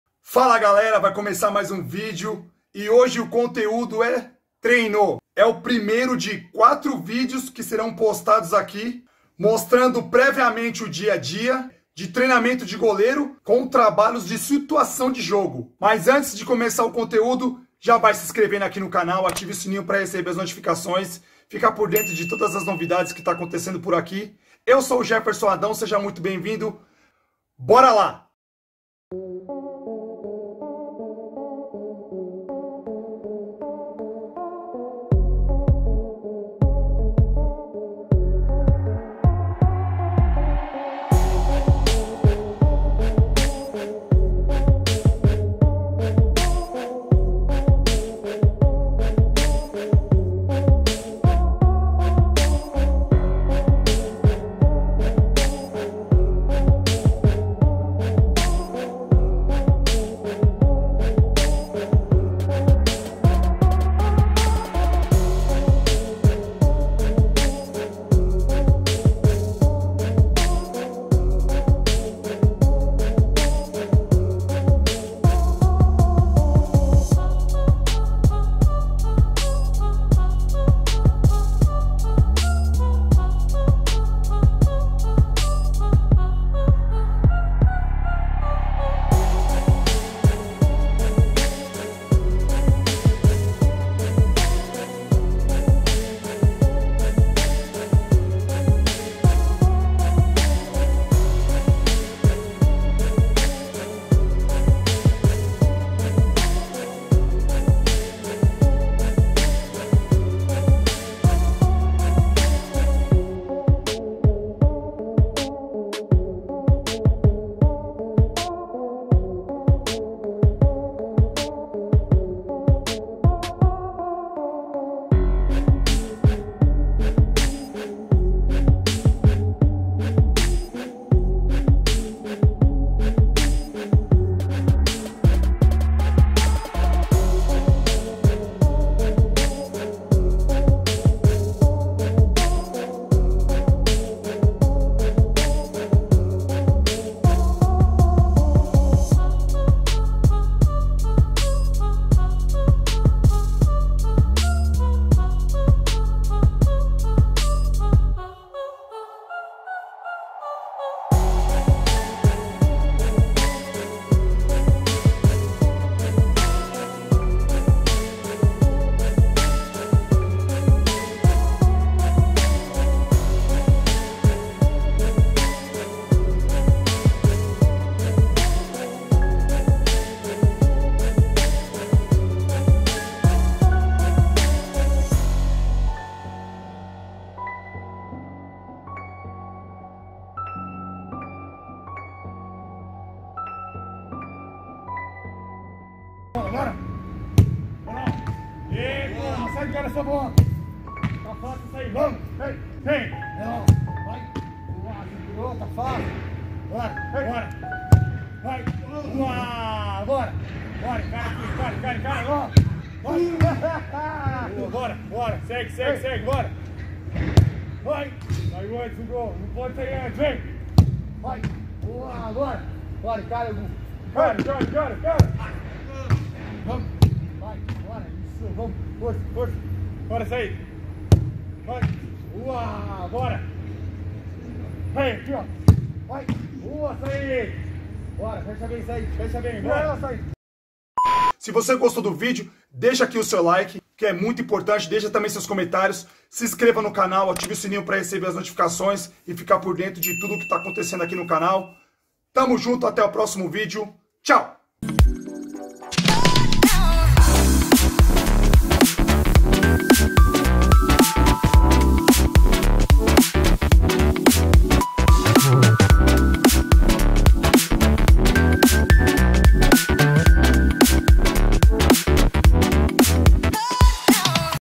TREINO DE SITUAÇÃO DE JOGO
TREINO DE CHUTE AO GOL EM DIVERSAS FORMAS, BOLA ROLANDO, BATE PRONTO, BOLA PARADA E TAMBÉM BOLAS PARADAS.

NESTE VIDEO FOI MONSTRADO TREINO DE SITUAÇÃO DE JOGO NA F.G, FOI PRIMEIRO VIDEO DE 4 CONTEÚDOS QUE IRÁ MOSTRAR O DIA DE TREINO DOS GOLEIROS